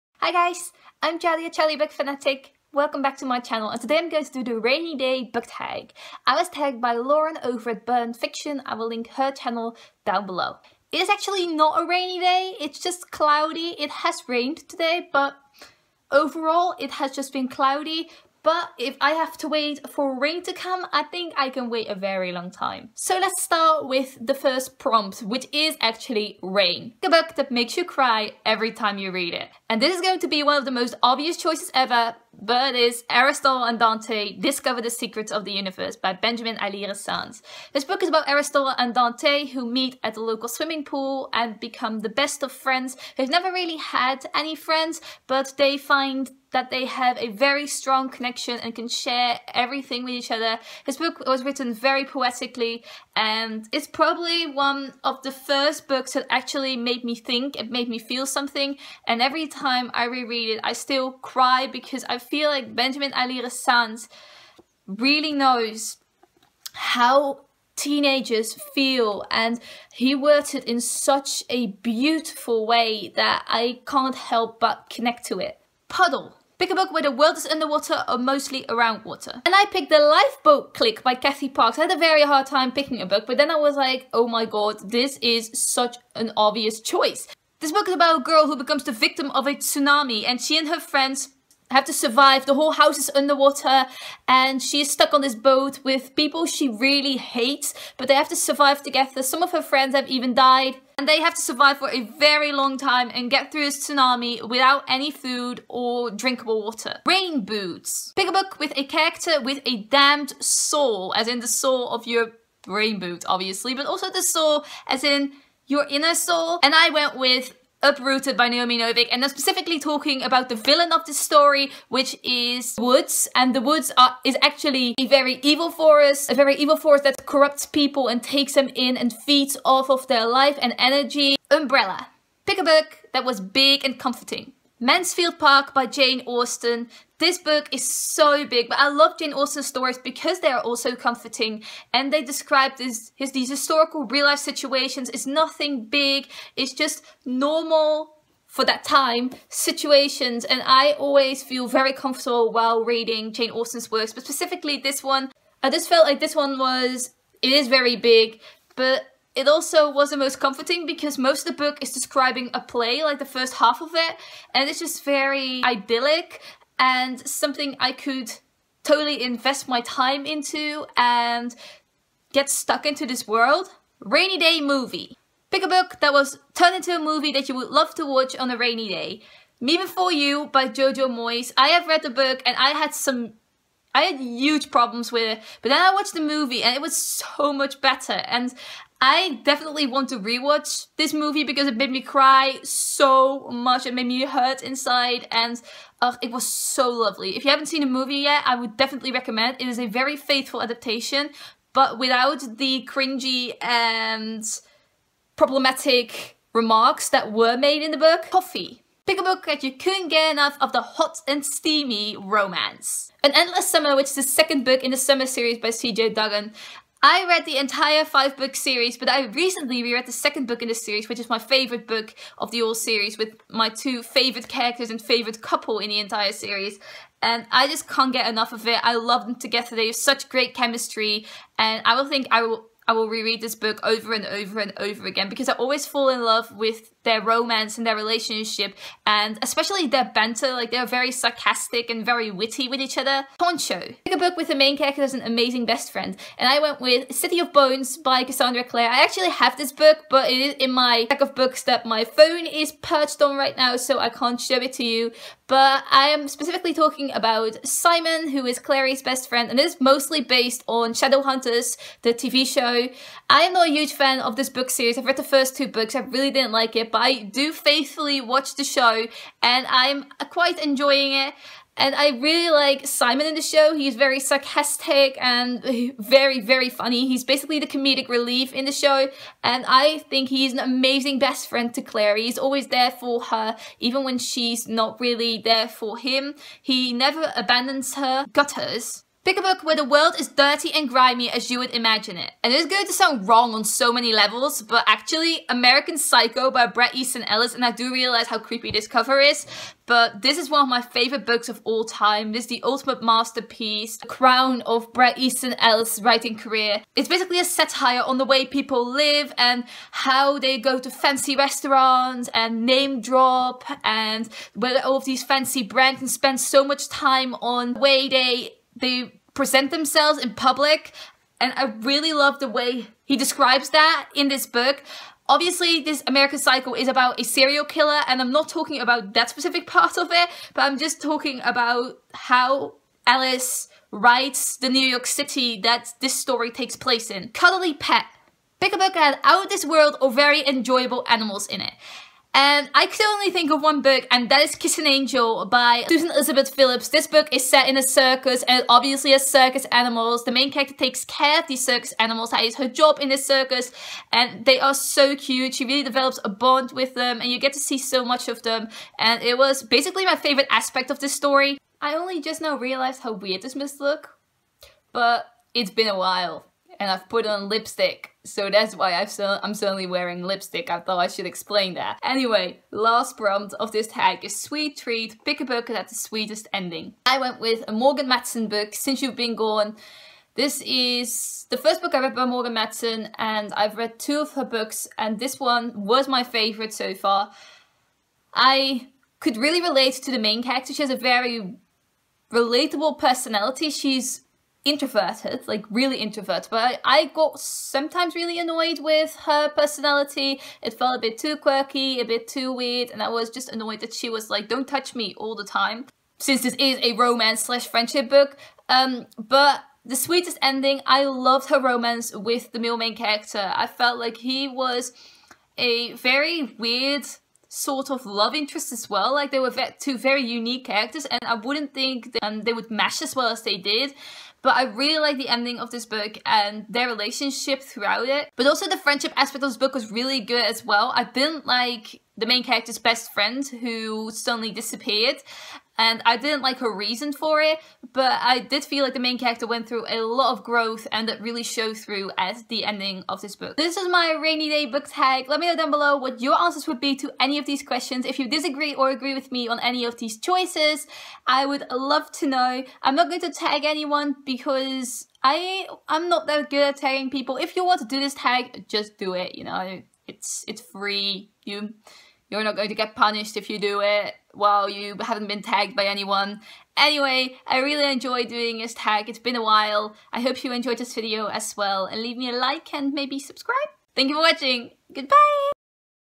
Rainy Day Book Tag | 2017
Books:
Aristotle and Dante Discover the Secrets of the Universe - Benjamin Alire Sáenz
The Lifeboat Clique - Kathy Parks
Uprooted - Naomi Novik
Mansfield Park - Jane Austen
Me Before You - Jojo Moyes
An Endless Summer - C.J. Duggan
City of Bones - Cassandra Clare
American Psycho - Bret Easton Ellis
Kiss an Angel - Susan Elizabeth Phillips
Since You've Been Gone - Morgan Matson